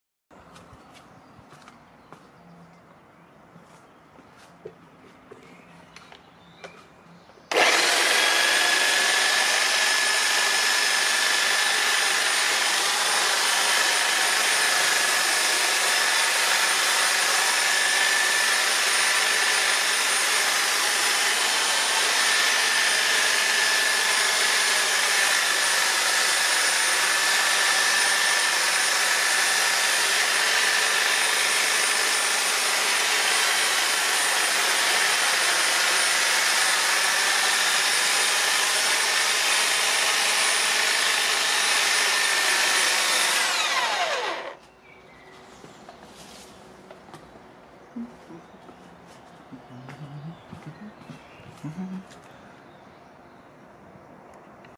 Hofer Heckenscheren Test – Hecke Schere – 18. VI. 2019 Panasonic Lumix fz82/83
Hofer Heckenscheren Test – 18. VI. 2019 Panasonic Lumix fz82/83

Eine Heckenschere ist ein Gartengerät und dient dazu, eine Hecke zu stutzen oder Gebüsch und feststängelige Pflanzen zurückzuschneiden.